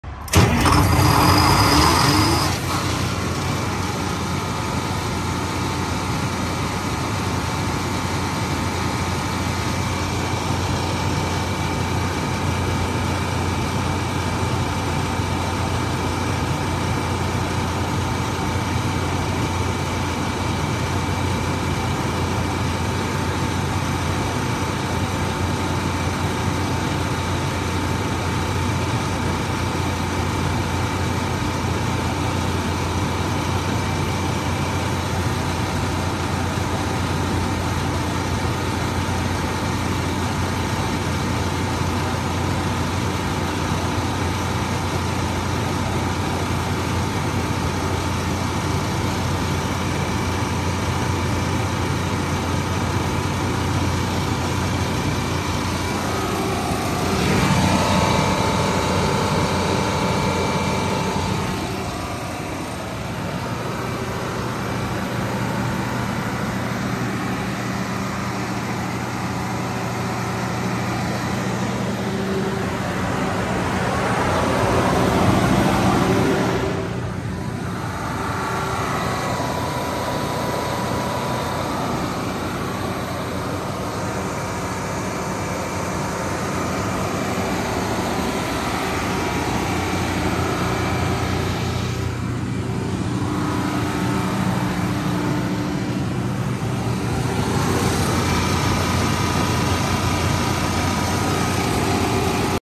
400kW Kohler diesel generator ID# 103465
400kW Kohler enclosed diesel generator set powered by a Detroit 8V92TT engine, 3 phase, 277/480V, 60Hz, 1800rpm with 128 hours.